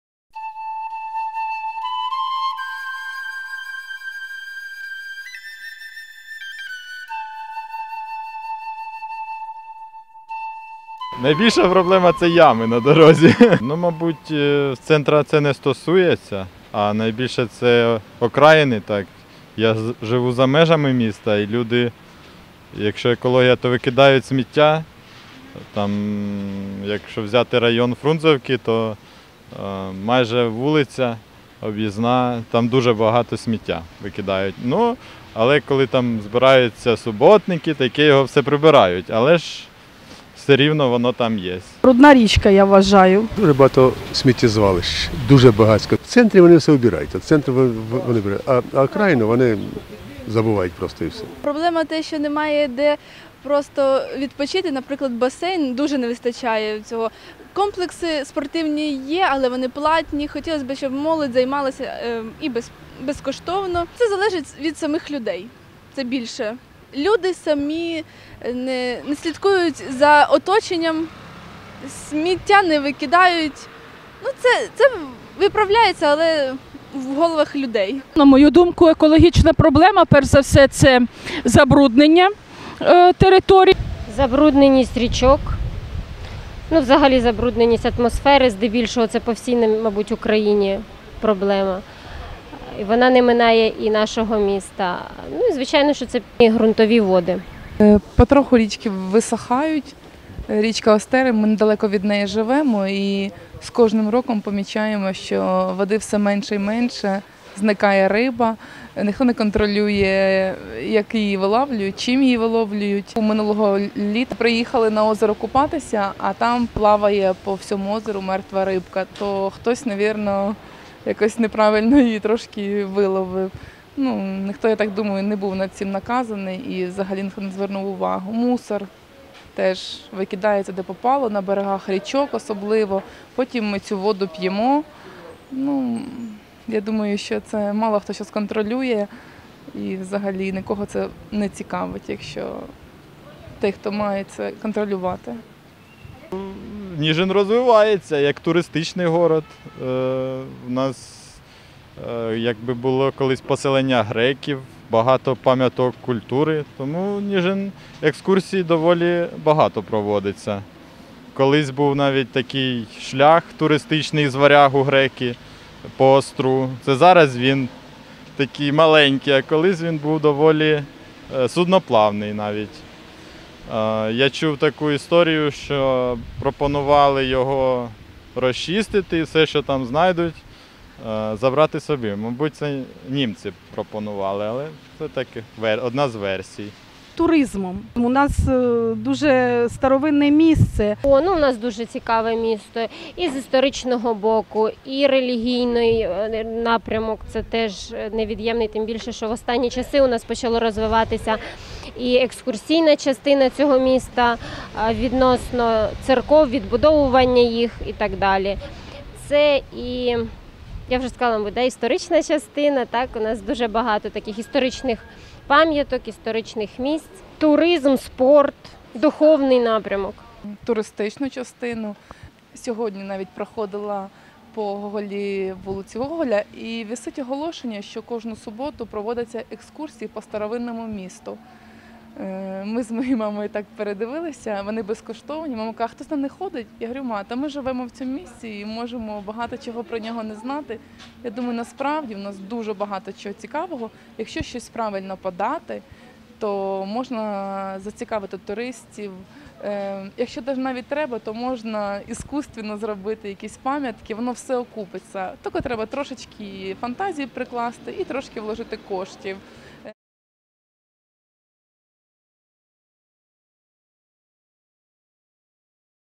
Residents of Nizhyn, Ukraine Reflect on Their City's Environmental Problems
Ukrainian NGO Ekoltava takes a microphone to the streets of Nizhyn, Urkraine, to ask residents about their biggest environmental concerns. The locals also speak about what they feel are the best ways for Nizhyn to make improvements going forward.